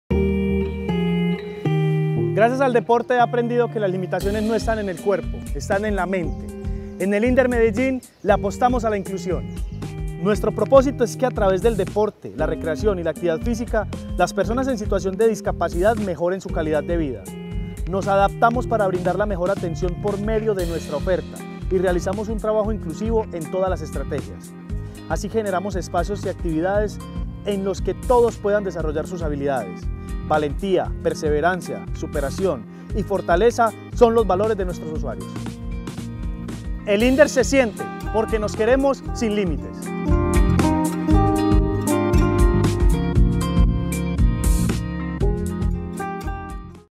Sin límites, nuestra apuesta por la población en situación de discapacidad
A través de nuestra estrategia Sin Límites acompañamos esta población en la ciudad. Mejoramos su calidad de vida con acompañamiento en deporte y actividad física